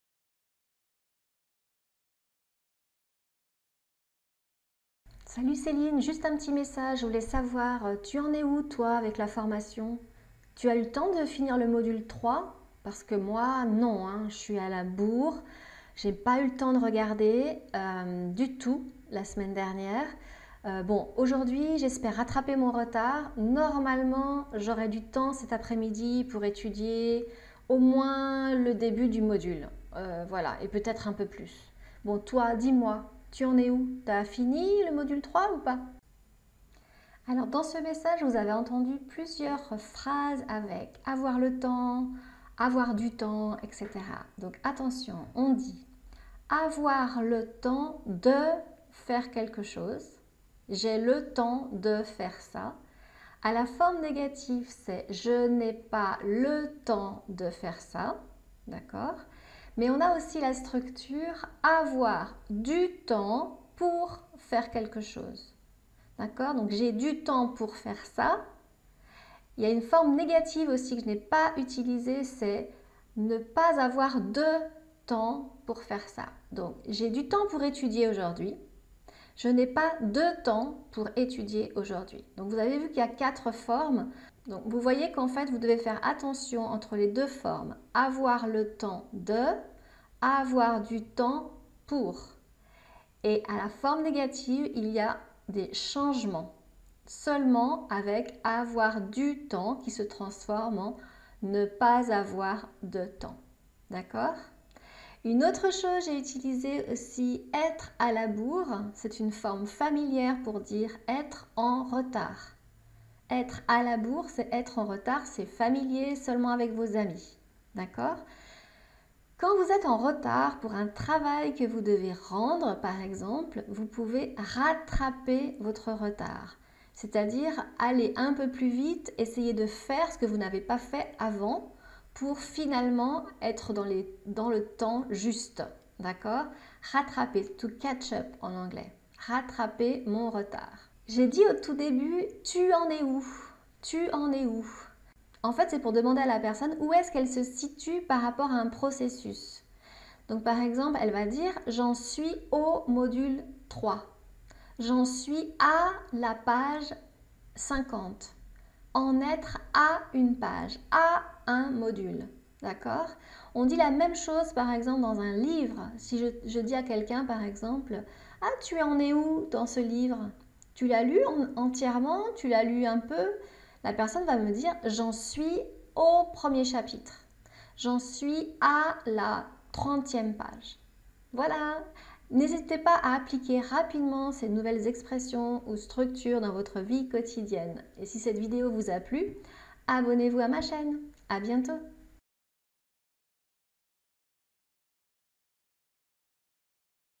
Vocabulaire pour parler de votre retard dans un projet ou une formation
Utilisez ces expressions très utiles pour parler de votre manque de temps et de votre retard dans une formation ou un projet.
Corrigez les erreurs fréquentes : avoir du temps ou avoir le temps ??

RECEVEZ DES CONTENUS GRATUITS : inscrivez-vous à mon infolettre (informations par email)